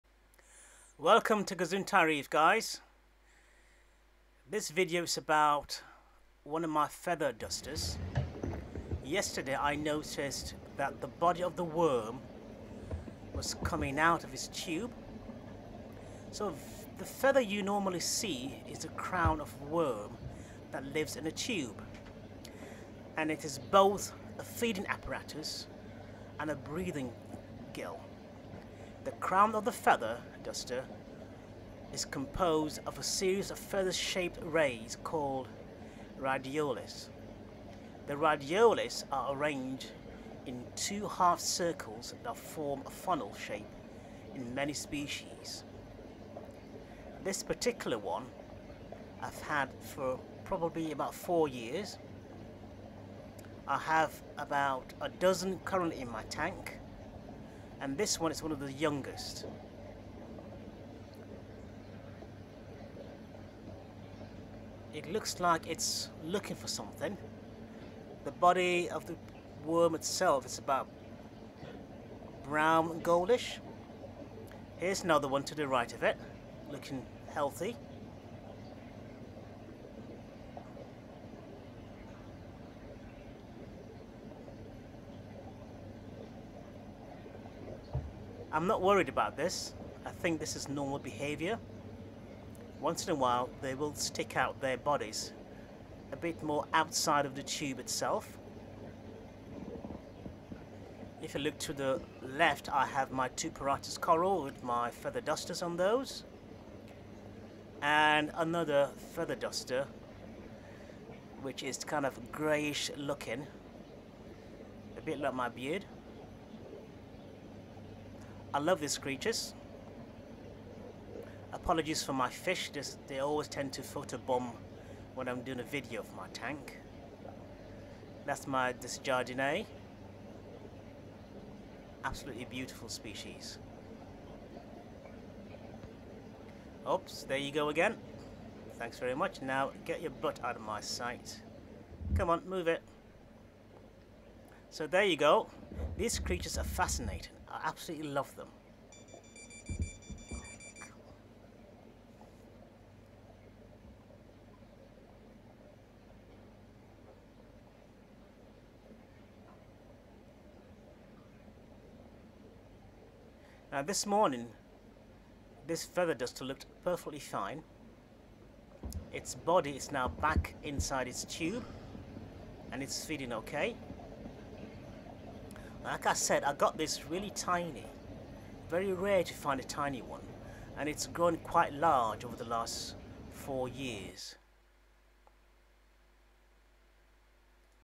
EPS2 GazuntaiReef - Have you ever seen a half naked Feather Duster's Tube worm body?
A rare look at a Feather Duster's tube worm body. This fantastic creatures are so amazing, I have them growing in my mixed reef tank.

Please excuse the video quality, I had to quickly grab my iPhone to capture this rare event.

System:
280 Gallon Custom Acrylic tank.
Two Algae Turf Scrubber (ATS) as primary filtration.
No water changes as standard.
Two Orphek Atlantik v4 Lights.